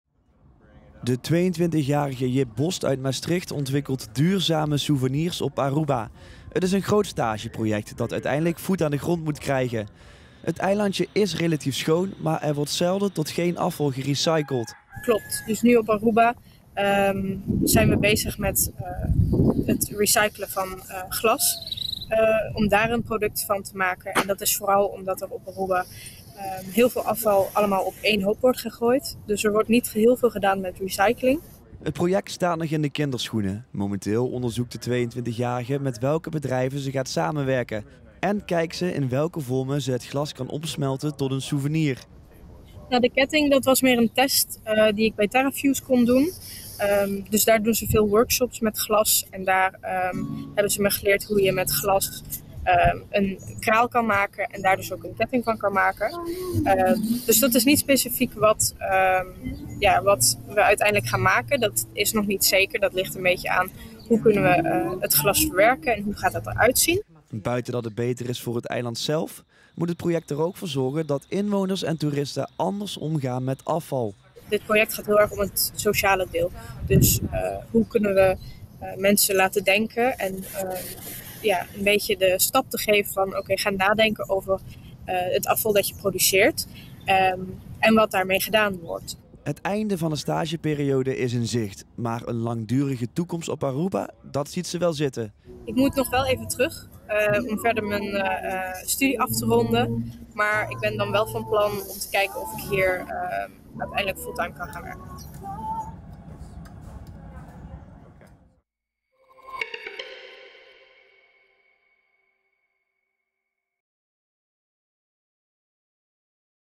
Jip Bost ontwikkelt duurzame souvenirs op Aruba voor stageproject♻️ | L1Nieuws
De 22-jarige Jip Bost uit Maastricht probeert de hoeveelheid afval op Aruba te verminderen. In plaats van dat weggegooid glas op een grote berg belandt, worden er binnenkort souvenirs van gemaakt.